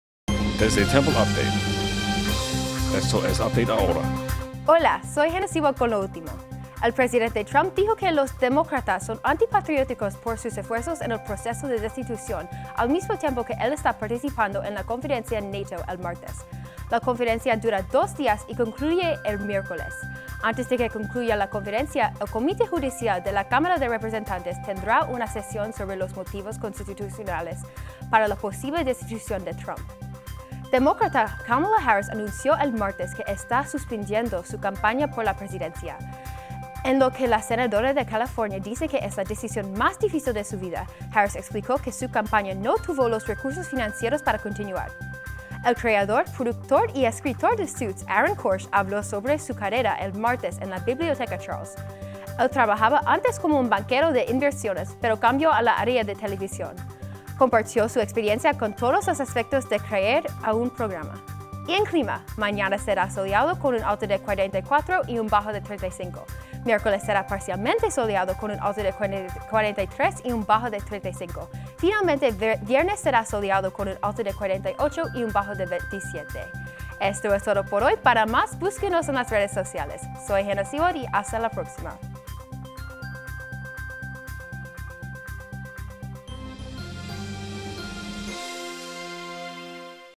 Update Ahora 3 Deciembre 2019
Desde el Cuarto de Noticias de Temple Update, esto es Update Ahora.
Aquí están las ultimas noticias:
-Reunión entre Trump y NATO
-Kamala Harris Deja la Elección
-Aaron Korsh en Temple